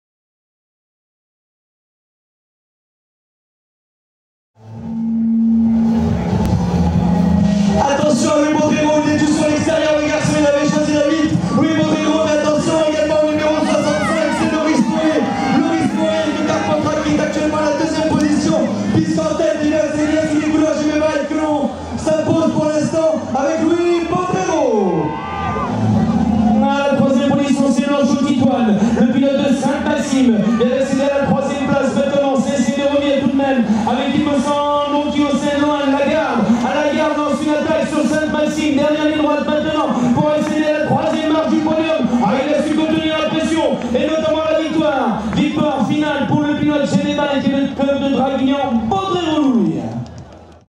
2013/04/21 National Bmx Sud Est Sainte Maxime Minime - Finale
BMX Race à SAINTE MAXIME le 20&21/04/2013
1ère manche du National BMX Sud Est
FINALE Catégorie Minime